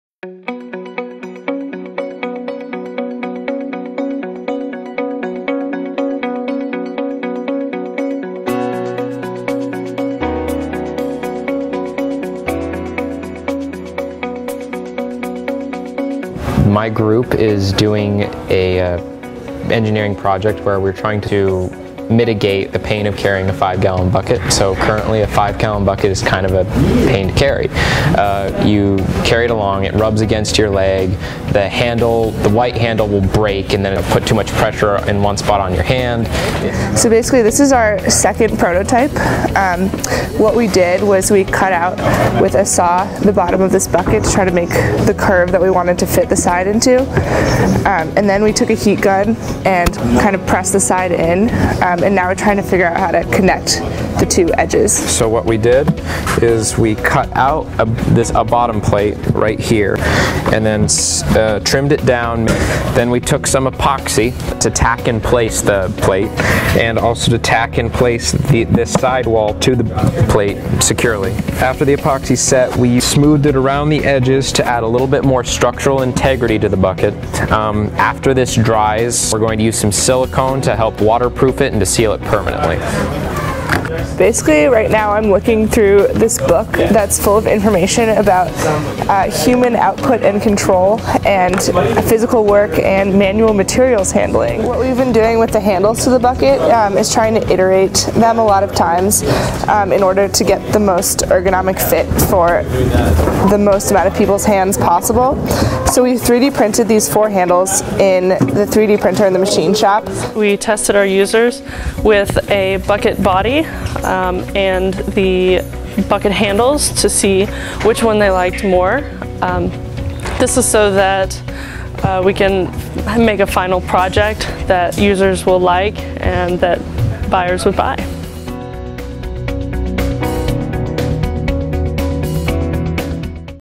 Designing a Better Bucket
Dartmouth engineering students designed a better bucket for their "Intro to Engineering" class, which challenged students to improve personal mobility.

Video edited by Alex Arcone.